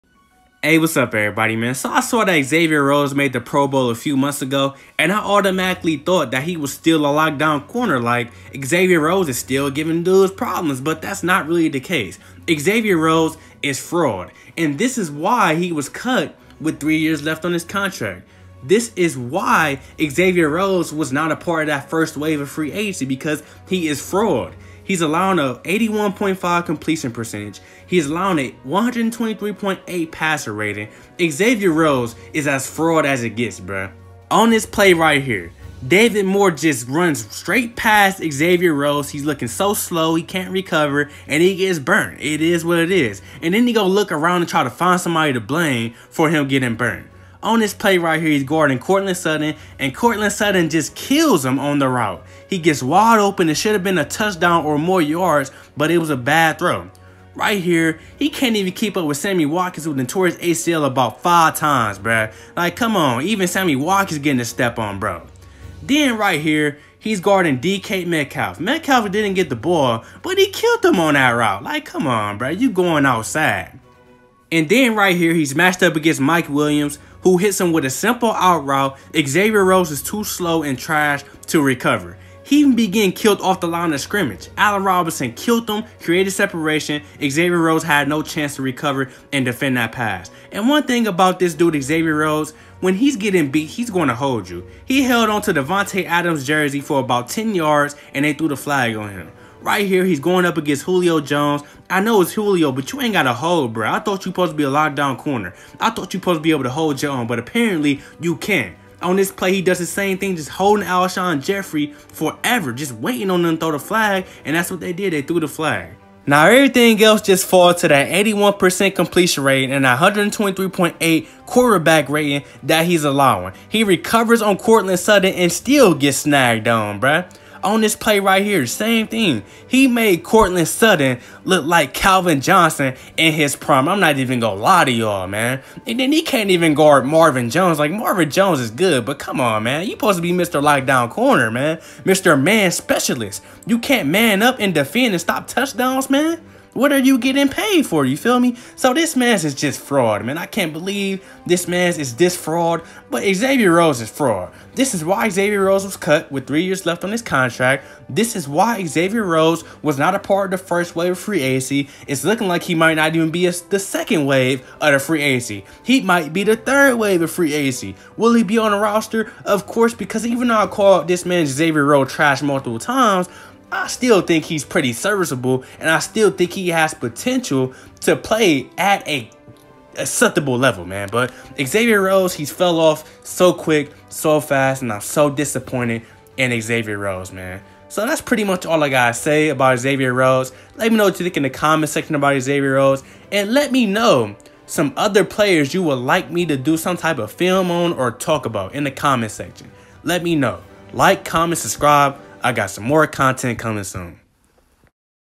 Xavier Rhodes Cut | here's why
Xavier Rhodes Cut | here's why. Xavier Rhodes is known as a man specialist lockdown corner and recently made the pro bowl. With that being said, people are wondering why the minnesota vikings cut Rhodes with 3 years left on his contract. People are also wondering why Xavier Rhodes has not been picked up yet in free agency. Her's why.